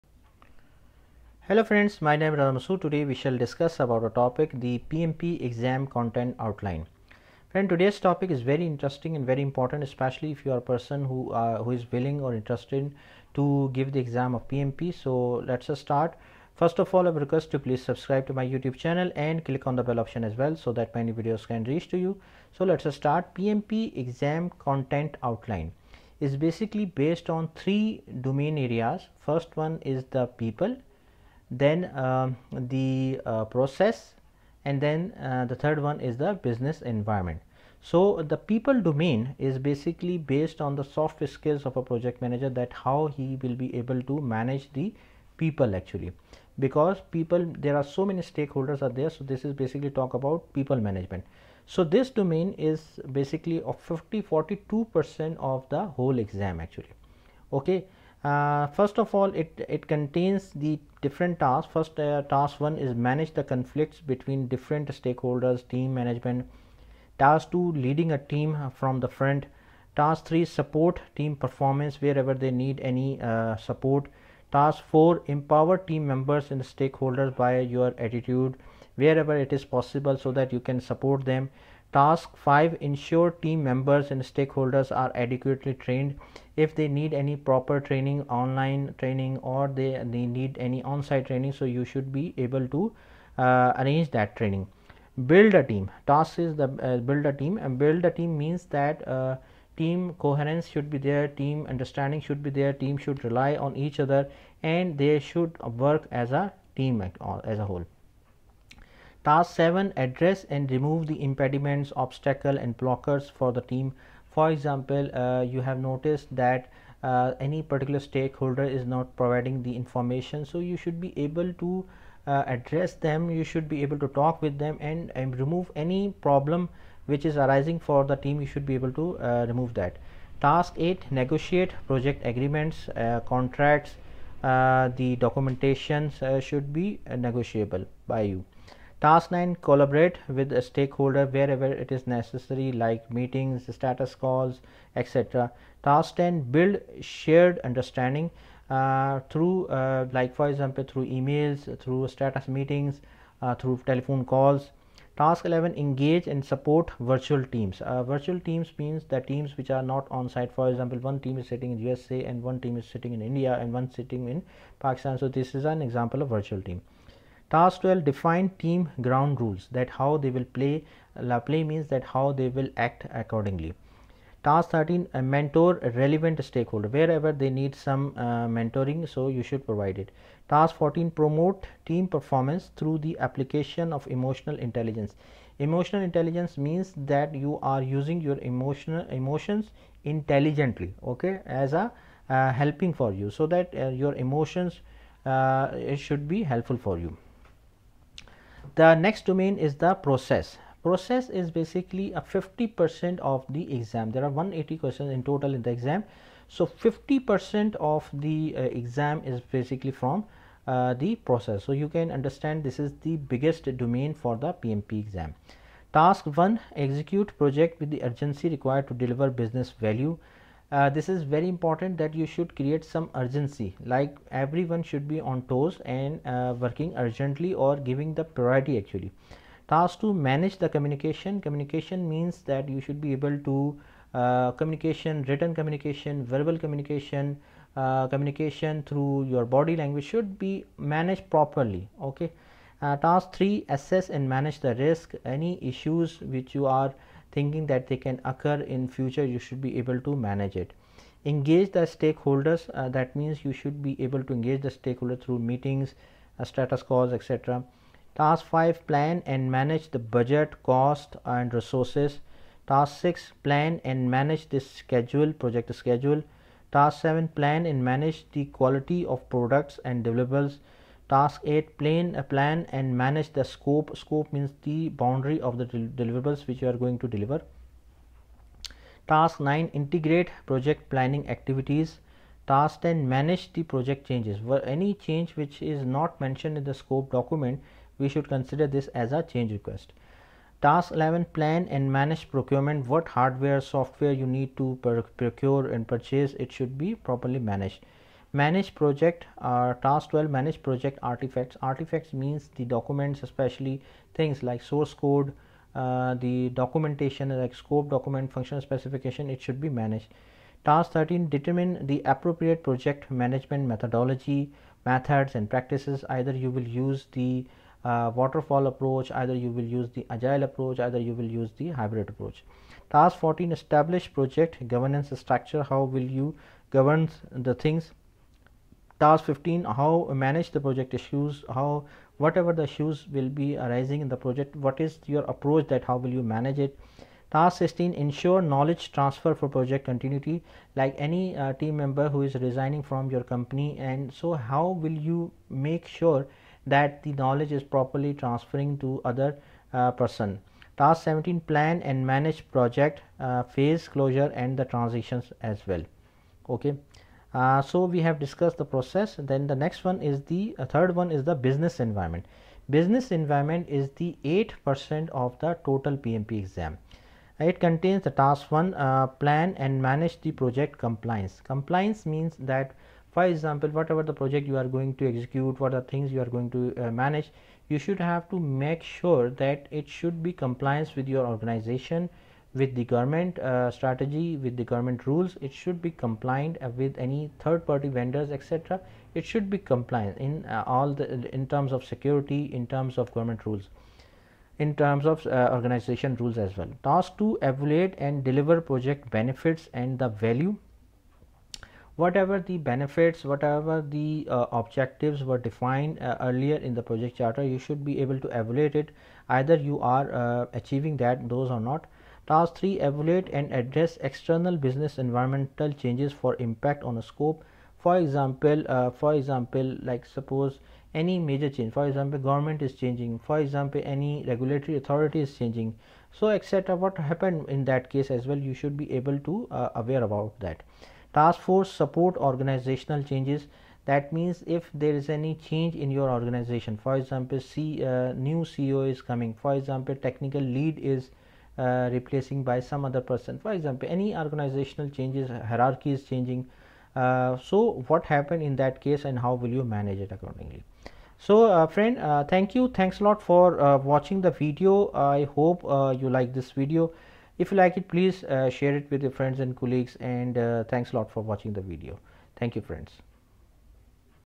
PMP Exam Content Outline
This video describes the PMP Exam content outline which consists of People, Process and Business Environment.